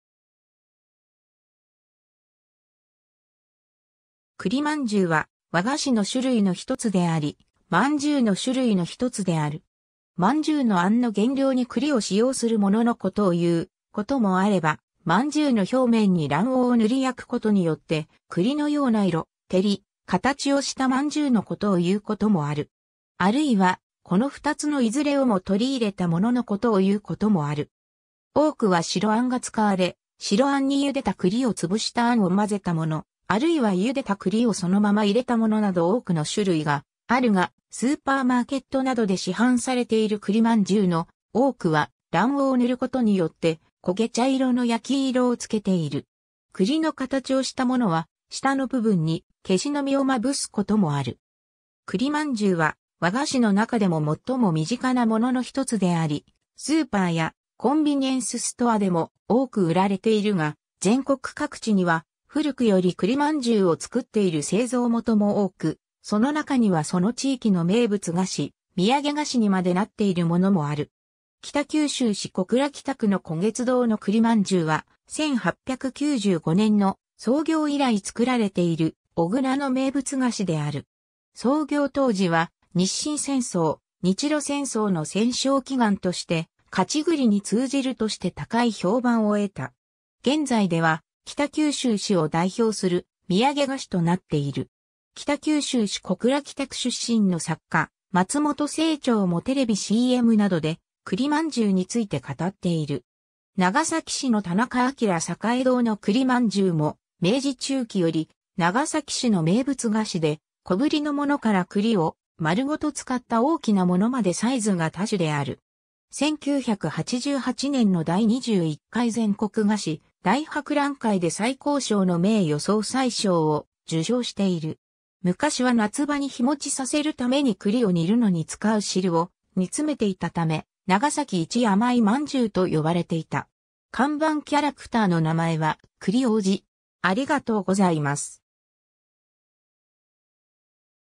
栗饅頭
栗饅頭（くりまんじゅう）は、和菓子の種類の一つであり、饅頭の種類の一つである。
饅頭の餡の原料に栗を使用するもののことを言うこともあれば、饅頭の表面に卵黄を塗り焼くことによって、栗のような色・照り・形をした饅頭のことを言うこともある。
あるいは、この二つのいずれをも取り入れたもののことを言うこともある。
 多くは白餡が使われ、白餡にゆでた栗をつぶした餡をまぜたもの、あるいはゆでた栗をそのまま入れたものなど多くの種類があるが、スーパーマーケットなどで市販されている栗饅頭の多くは、卵黄を塗ることによって焦げ茶色の焼き色をつけている。
栗の形をしたものは下の部分にケシの実をまぶすこともある。
 栗饅頭は、和菓子の中でも最も身近なものの一つであり、スーパーやコンビニエンスストアでも多く売られているが、全国各地には古くより栗饅頭を作っている製造元も多く、その中にはその地域の名物菓子、土産菓子にまでなっているものもある。
 北九州市小倉北区の湖月堂（こげつどう）の栗饅頭は、1895年（明治28年）の創業以来作られている小倉の名物菓子である。
創業当時は日清戦争・日露戦争の戦勝祈願として、「勝ち栗」に通じるとして高い評判を得た。
現在では北九州市を代表する土産菓子となっている。
北九州市小倉北区出身の作家・松本清張もテレビCMなどで栗饅頭について語っている。
 長崎市の田中旭栄堂（たなかきょくえいどう）の栗饅頭も、明治中期より長崎市の名物菓子で、小ぶりのものから栗を丸ごと使った大きなものまでサイズが多種である。
1988年の第21回全国菓子大博覧会（松江市）で最高賞の名誉総裁賞を受賞している。
昔は夏場に日持ちさせるために栗を煮るのにつかう汁を煮詰めていたため、「長崎一甘い饅頭」と呼ばれていた。
看板キャラクターの名前は「栗王子」。